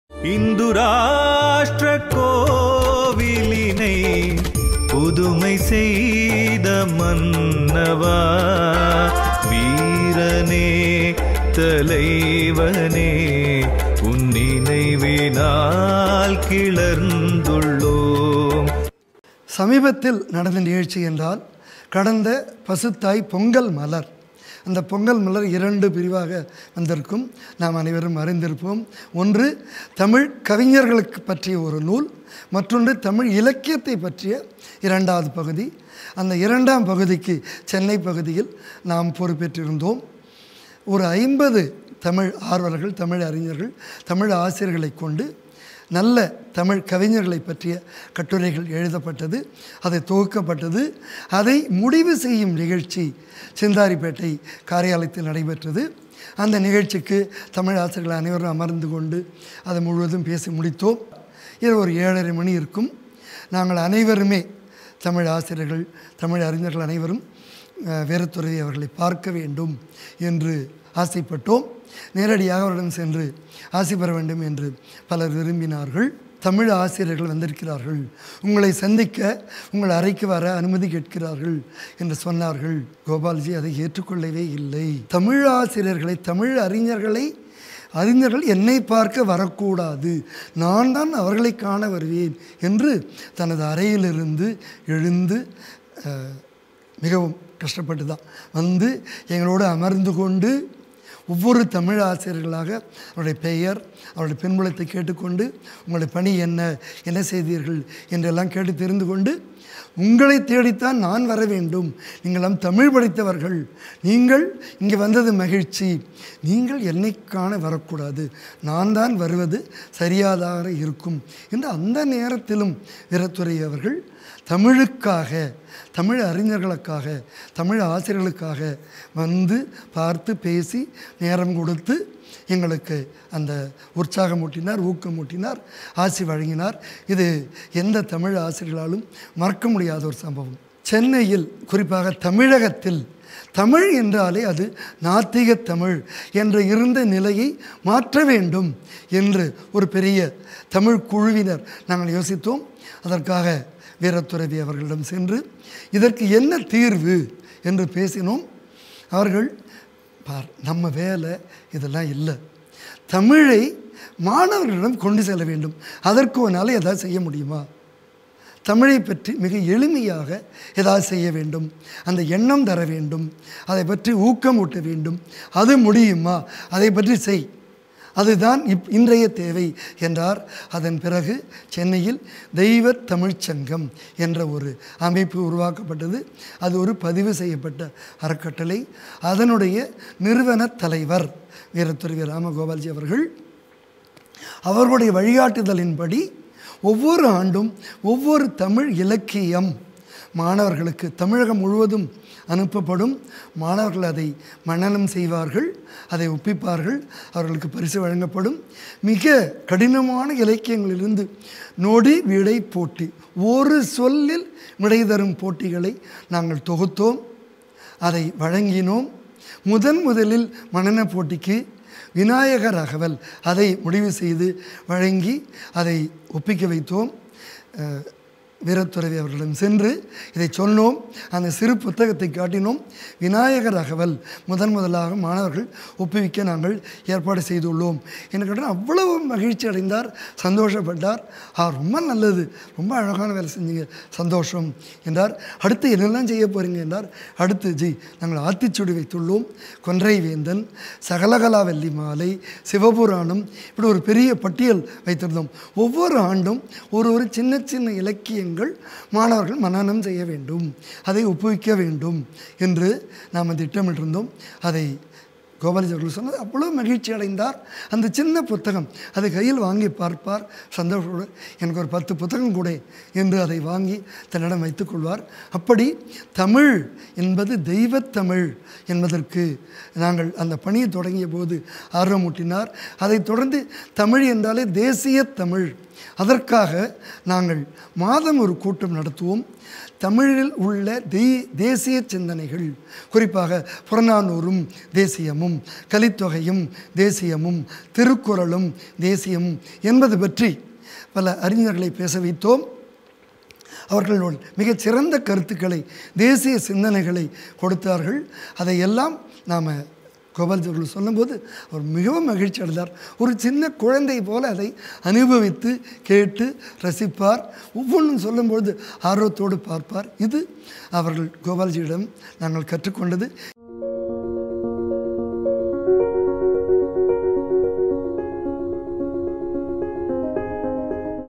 உன் நினைவினால் கிளர்ந்துள்ளோம் - பாகம் 75 - கு. வேங்கடேசன்
இராம கோபாலன் அவர்களின் ஆசியால் அவருடன் தொடர்பு ஏற்பட்டவர்கள் அவரைப் பற்றிய அனுபவங்களை இந்நிகழ்ச்சியில் பகிர்ந்து கொள்கிறார்கள்.

இப்பகுதியில், தமிழய்யா கு. வேங்கடேசன் அவர்கள் வீரத்துறவியுடனான தன் நினைவுகளைப் பகிர்ந்து கொள்கிறார்.